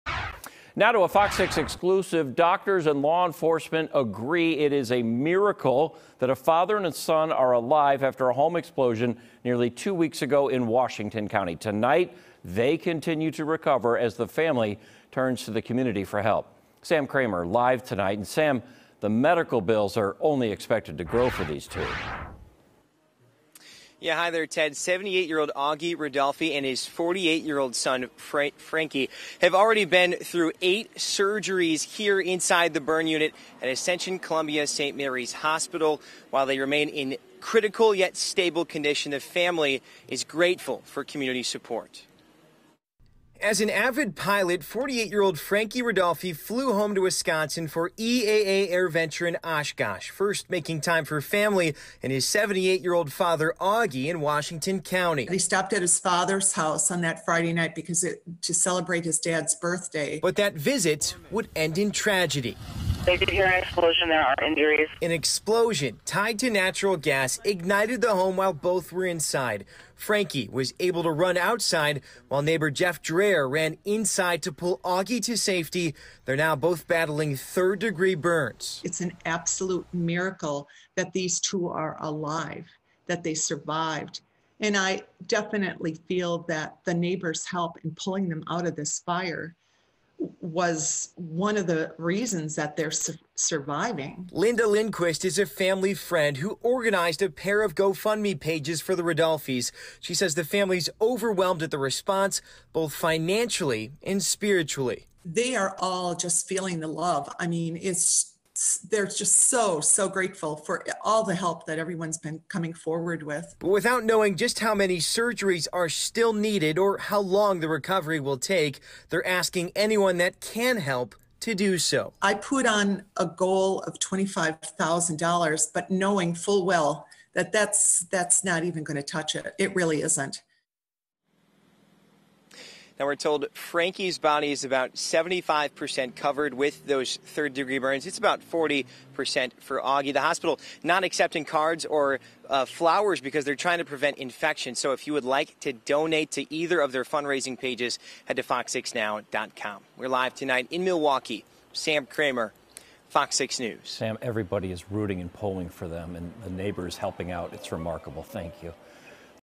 Community rallies around Barton burn victims | FOX6 News Milwaukee
Doctors and law enforcement agree it's a miracle a father and son are alive after a home explosion on July 23 in Washington County. They continue to recover as the family turns to the community for help, with their medical bills only expected to grow.